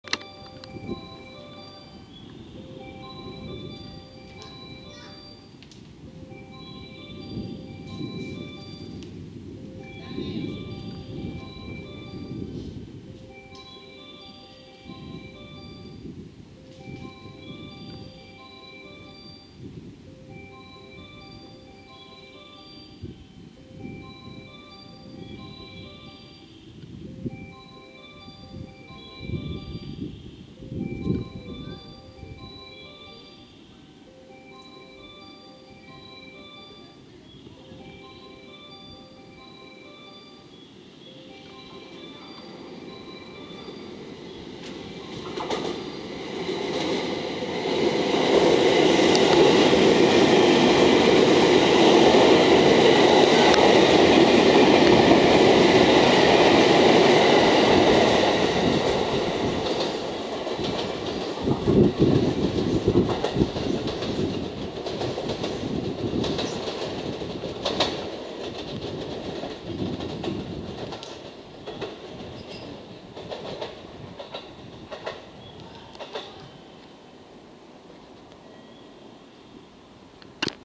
備後地区に来たキティー Hello Kitty HARUKA 回9081M 山陽本線 下り 回送列車 271系 HA654 HA653編成 JR西日本 東福山 2022.8.7 05509
備後地区に来たキティー Hello Kitty HARUKA 回9081M 山陽本線 下り 回送列車 271系 HA654 HA653編成 JR西日本 東福山

271系 HA654 HA653編成